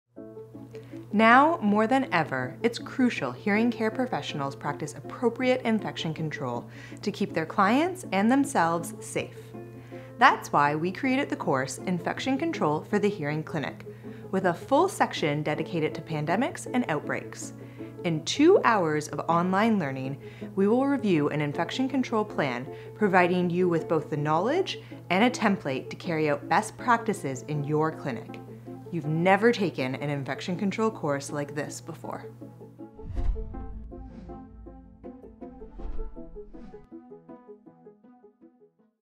Are you ready to master infection control in your hearing clinic? | Course Trailer
Get a glimpse of the most comprehensive infection control training program for hearing care professionals.

This two-hour program includes a special section dedicated to pandemics and outbreaks, infection control best practices for hearing care, a full review of an infection control plan with a downloadable template, product reviews, ultra high definition video instruction, and a printable certificate upon completion.